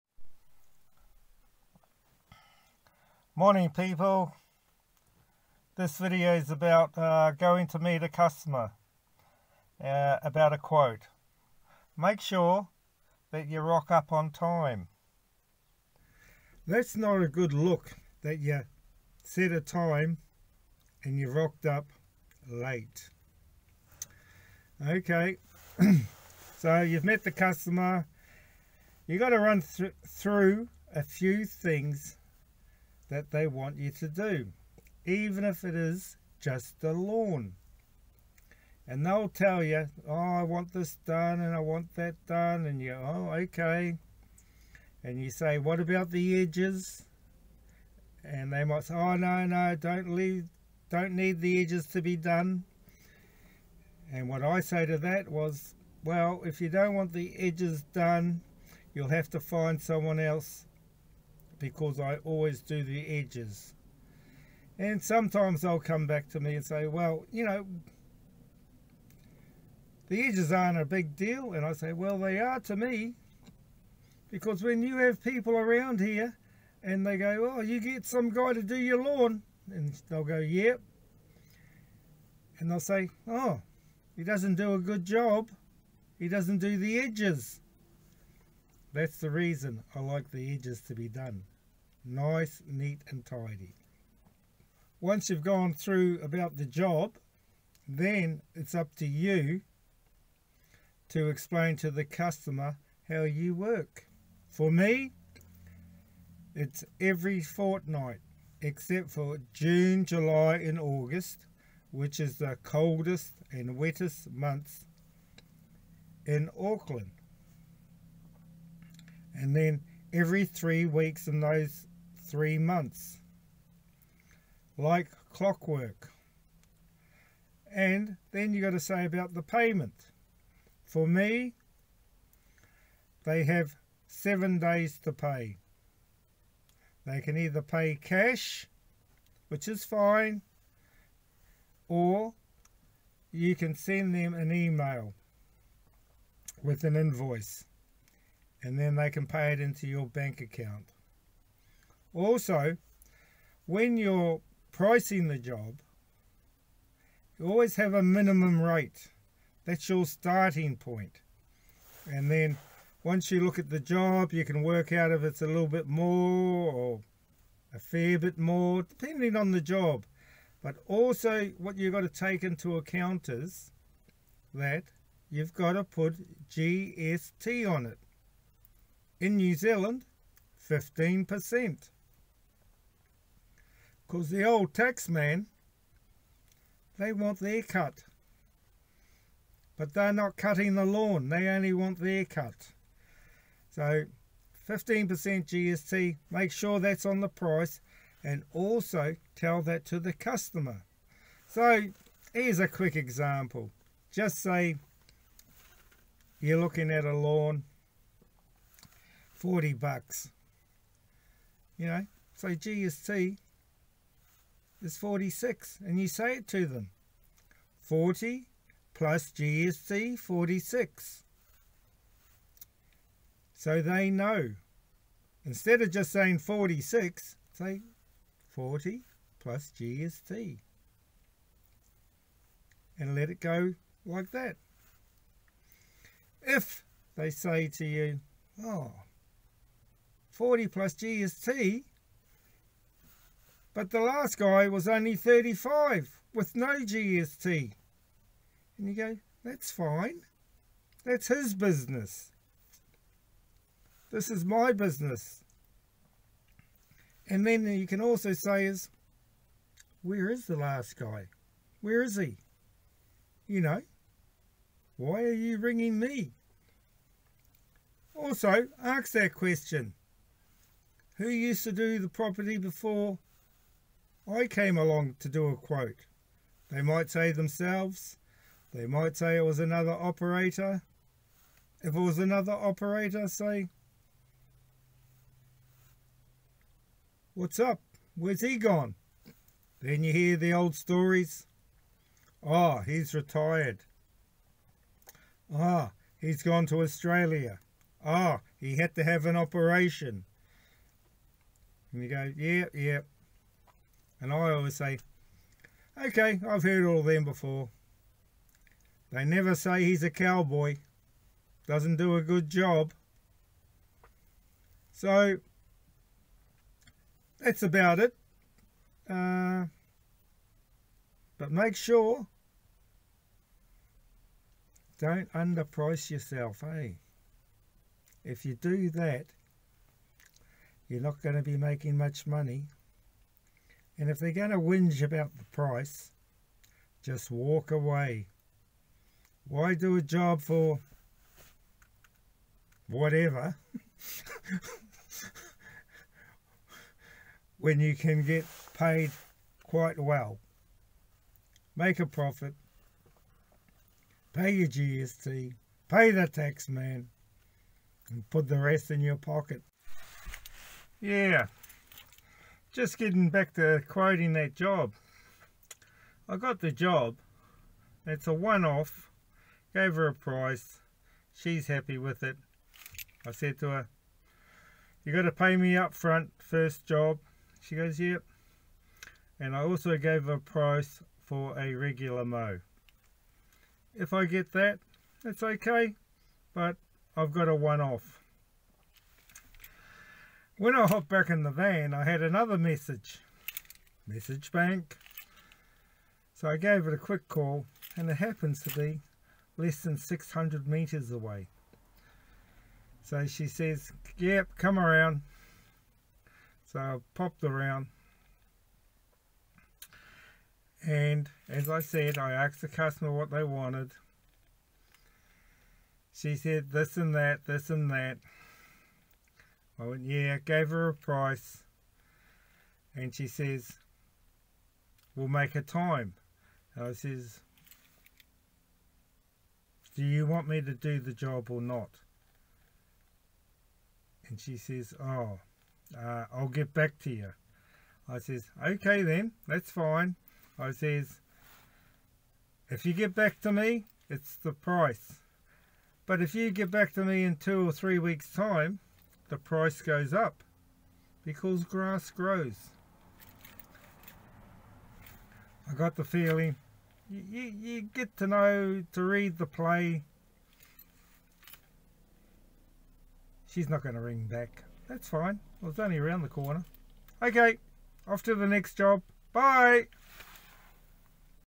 Basics of Quoting
A few handy hints on meeting a customer on time.